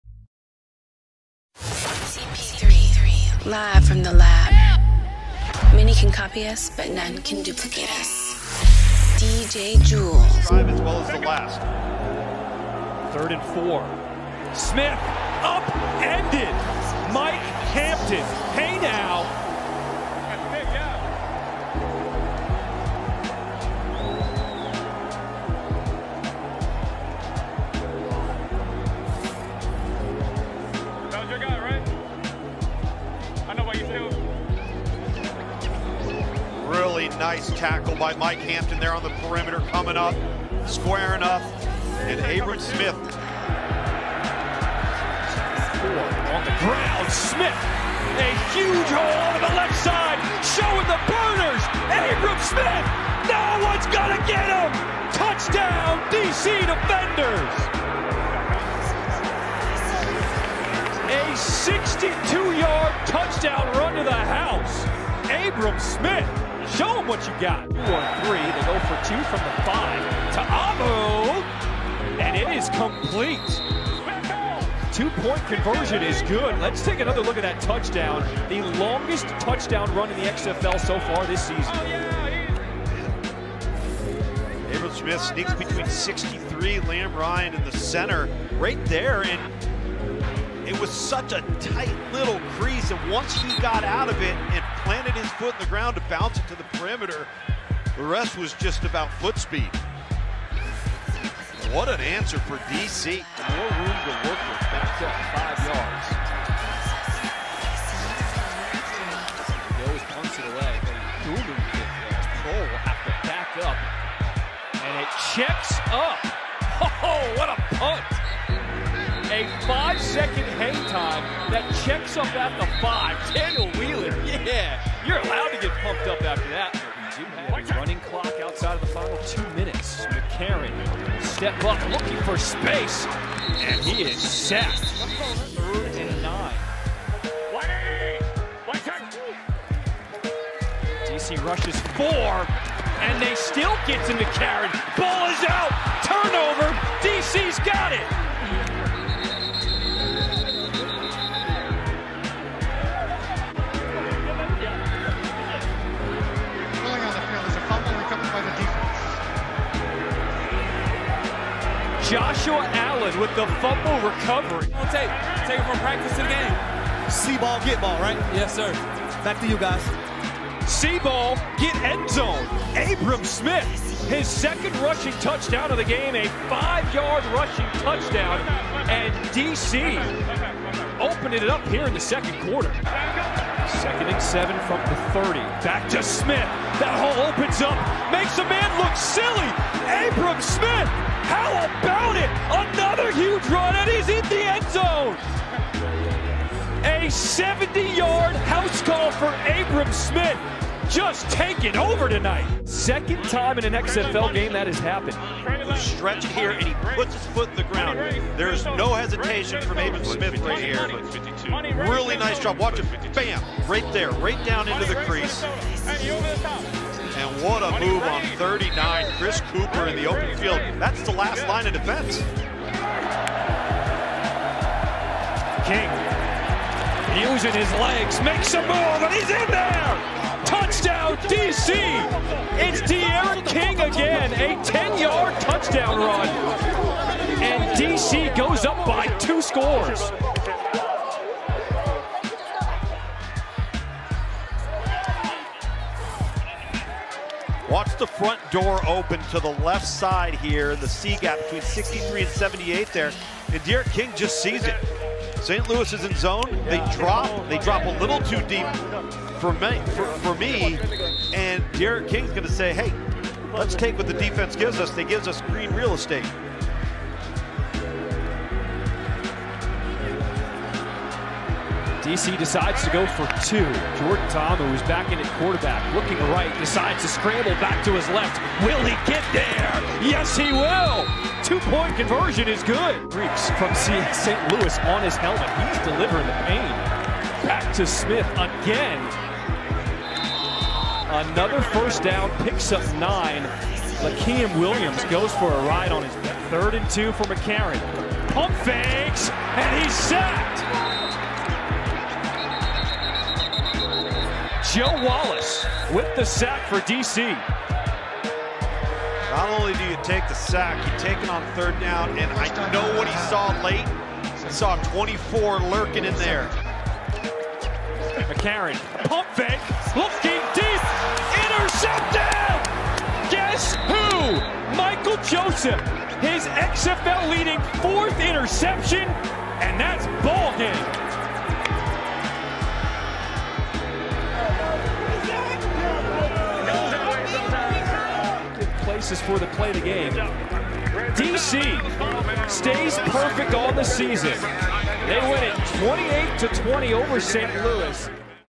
SportsOTHP Highlights Defenders (5-0) win 28-20 over StLouis (3-2) XFL 03.20.23
ICYMI : D.C. Defenders improved to (5-0) defeated St. Louis Battlehawks (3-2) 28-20. The Defenders scored all if their TD’s on th ground, led by league leading rusher AbramSmith. Smith rushed 23 times for 218 yards and 3 Td’s. The defense stood up showed up and showed out. Led by MichealJoseph who got his league leading 4th int to end the threat late in the game.
Video courtesy: ESPN